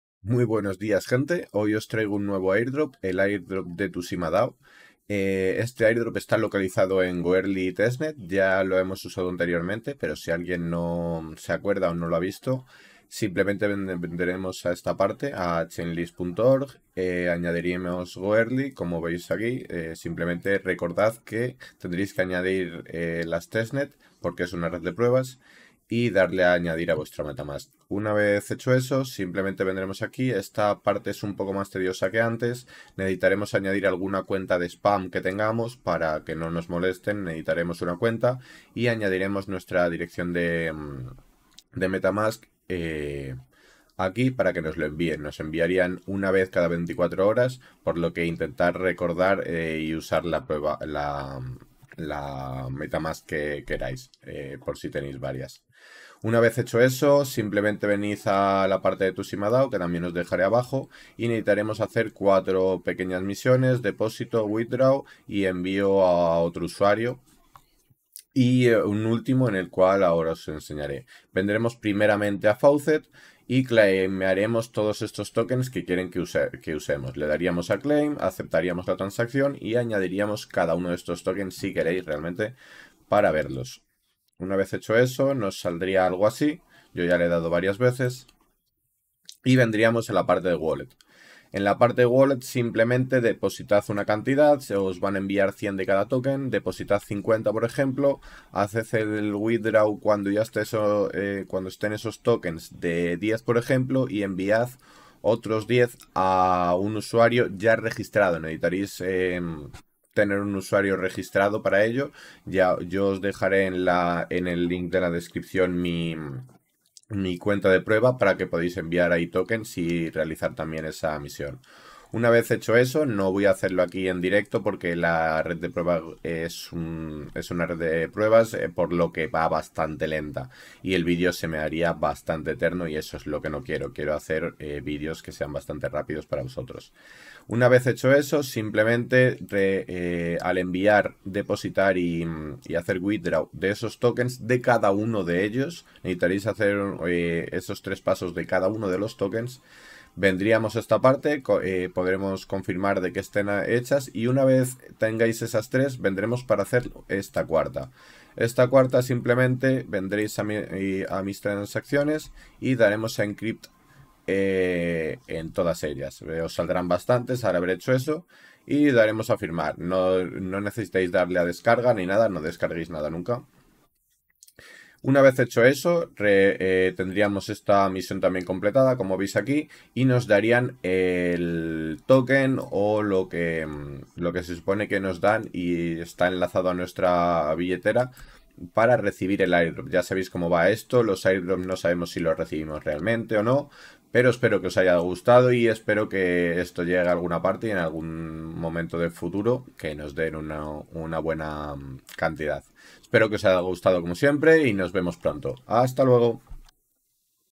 NUEVO AIRDROP DE TUSIMA - CALIFICA EN 3 MINUTOS 2023
Nuevo airdrop disponible, bastante rápido (Sí goerli tiene buen dia xD)